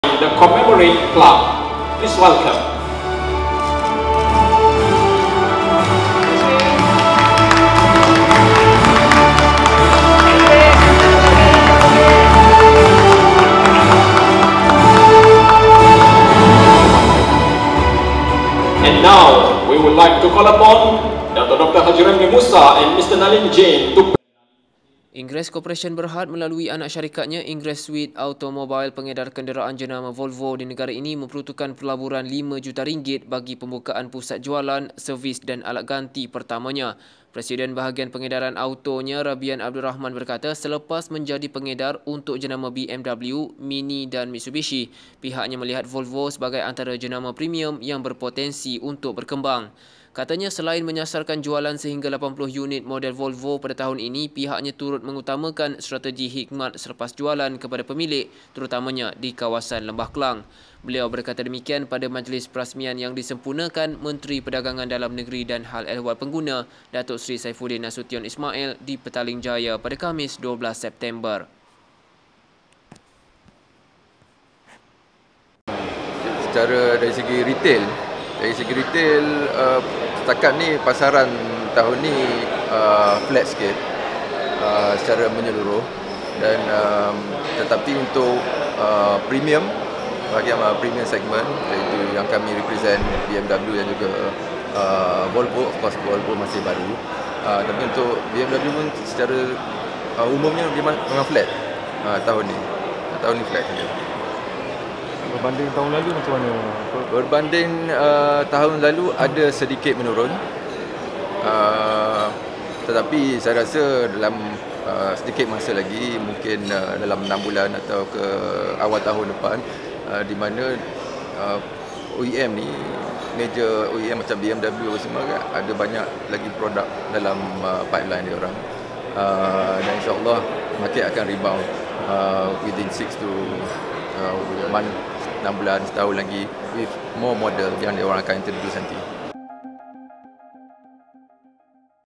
Volvo ada potensi
Ingress Corporation Berhad melalui anak syarikatnya Ingress Swede Automobile, pengedar kenderaan jenama Volvo di negara ini memperuntukkan pelaburan RM5 juta bagi pembukaan pusat jualan, servis dan alat ganti (3S) pertamanya.

Presiden Bahagian Pengedaran Autonya, Rabian Ab Rahman berkata, selepas menjadi pengedar untuk jenama BMW, Mini dan Mitsubishi, pihaknya melihat Volvo sebagai antara jenama premium yang berpotensi untuk berkembang.